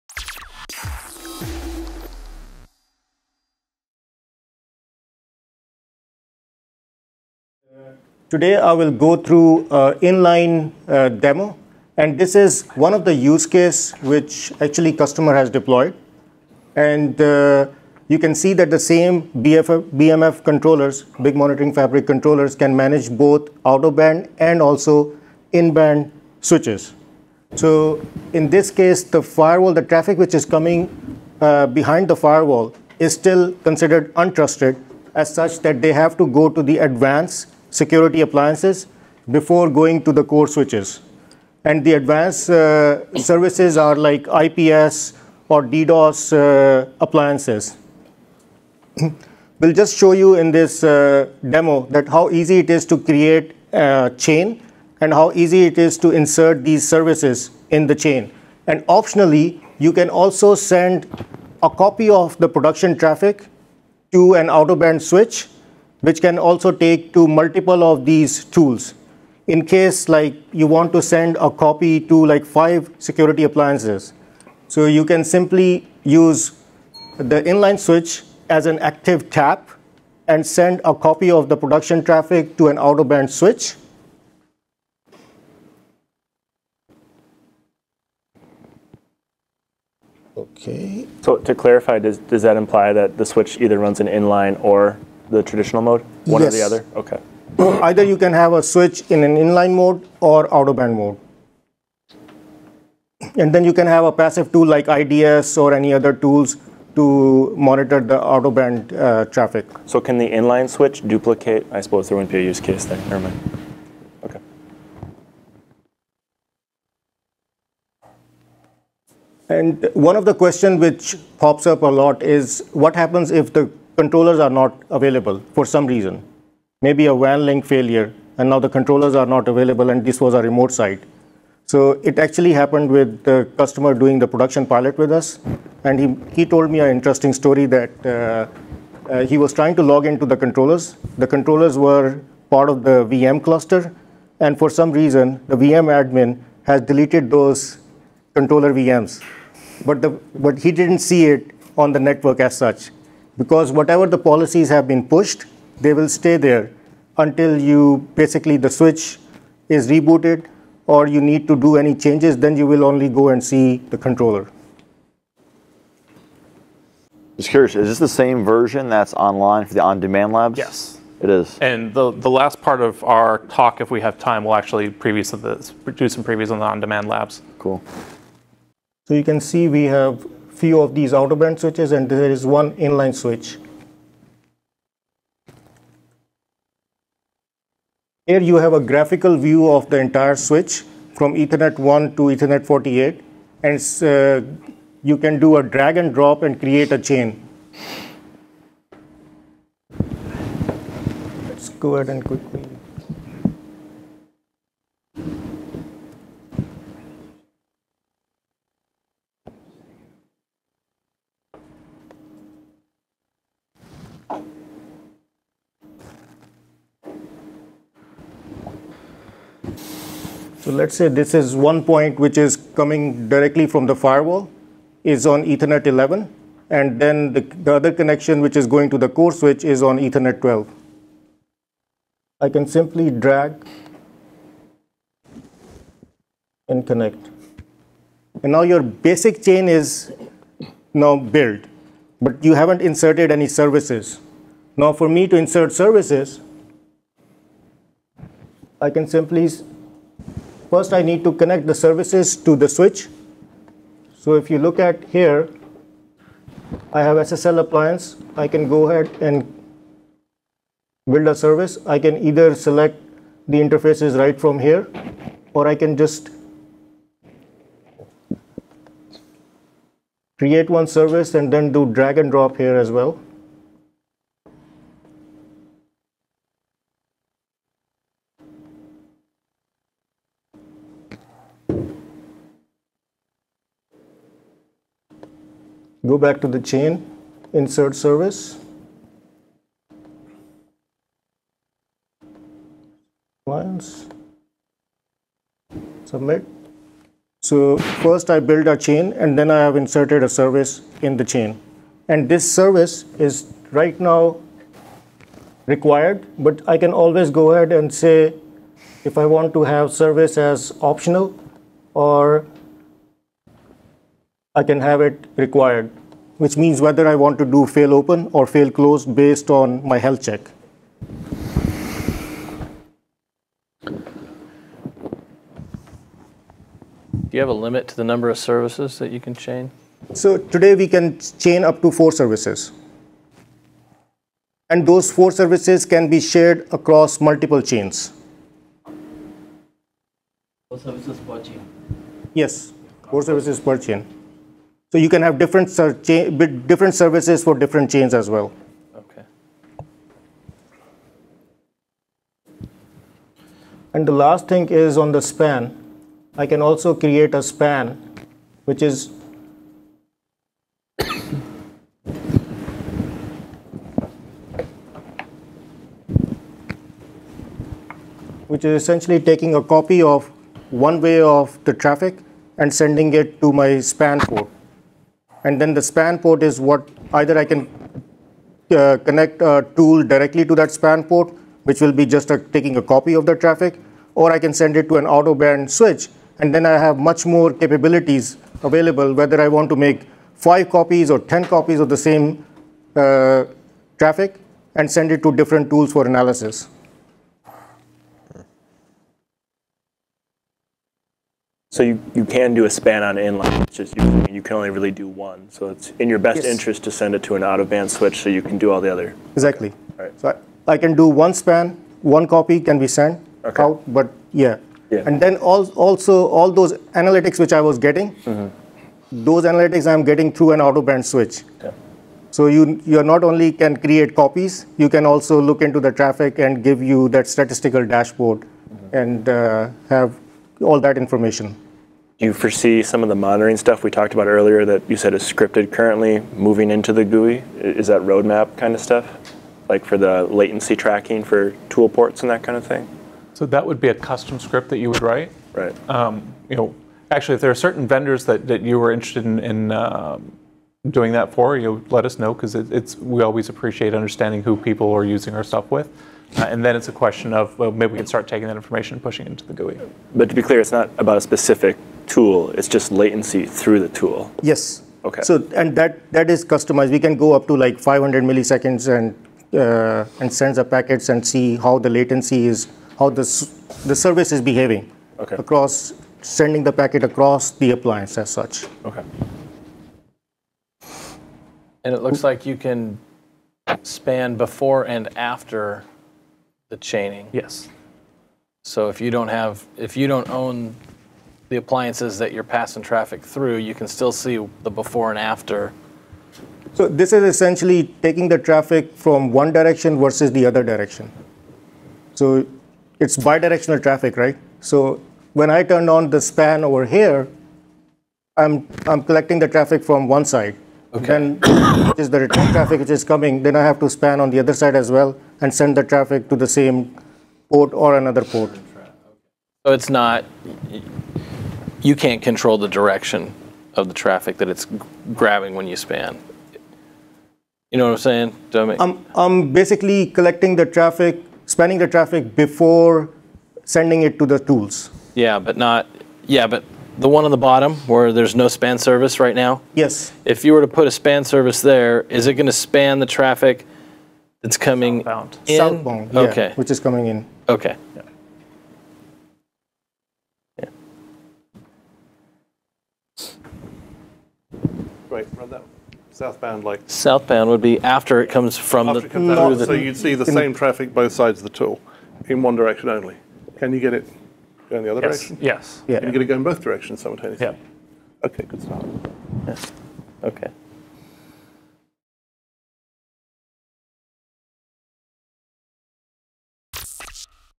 Big Switch Networks Big Monitoring Fabric Inline Mode Demo
Syed Ghayur, Sales Engineer, demonstrates the inline mode of Big Monitoring Fabric and showcases some of the differences between this solution and the out-of-band mode. Recorded at Networking Field Day 11 on January 20, 2016.